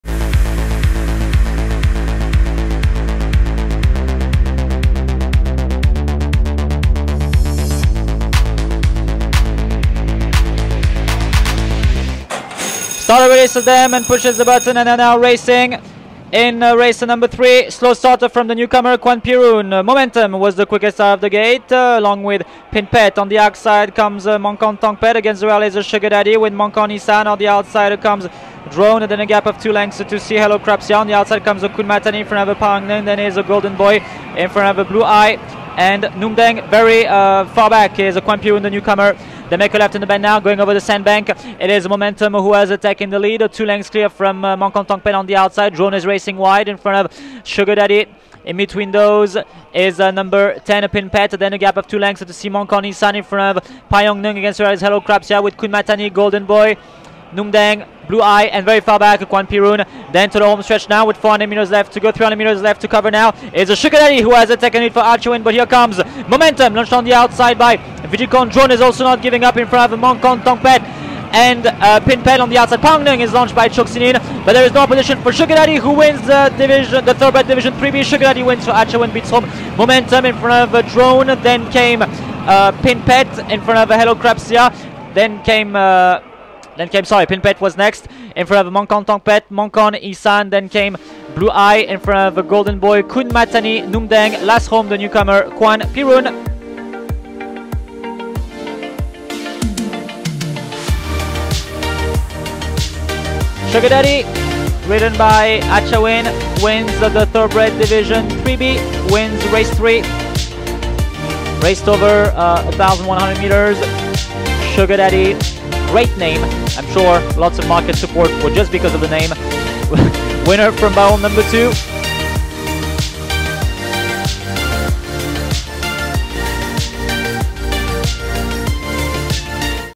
SUGAR DADDY wins! - 27/10/2024 - Race 3 - Thoroughbred Division 3B
Bangkok, Thailand - SUGAR DADDY wins! - 27/10/2024 - Race 3 - Thoroughbred Division 3B

Track Going - Good To Soft